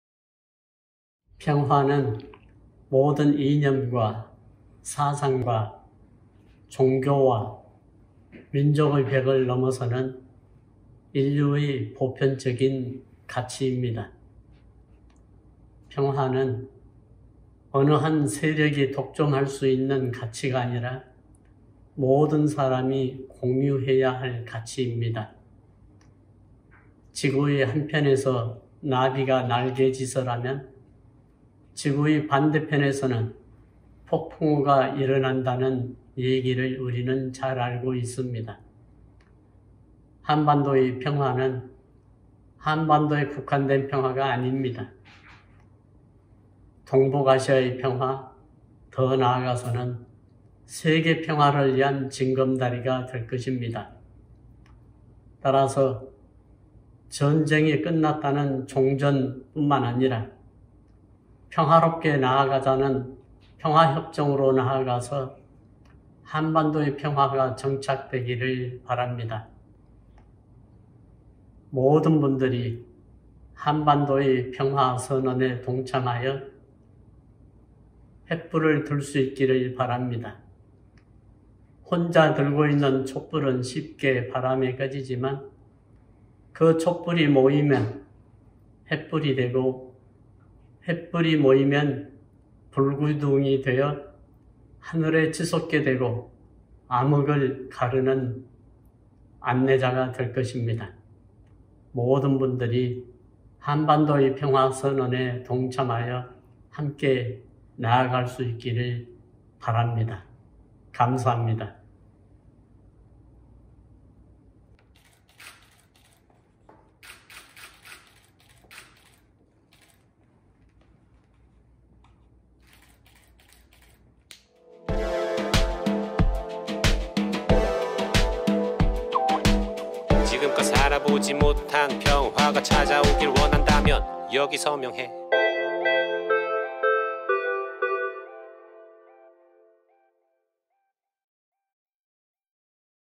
김희중 대주교 "서명 동참하여 함께 평화의 불기둥을 세웁시다"
한반도 종전과 평화를 위한 Korea Peace Appeal 전 세계 서명운동에는 많은 분들이 함께하고 있어요. 7대 종단도 하나되어 시민들의 서명 동참을 호소했습니다. 한반도 종전 평화 캠페인 명예대표이신 김희중 대주교(한국 천주교 주교회의 의장)님을 만나보았습니다.

🕊한반도 종전과 평화를 위한 전 세계 1억 명 서명운동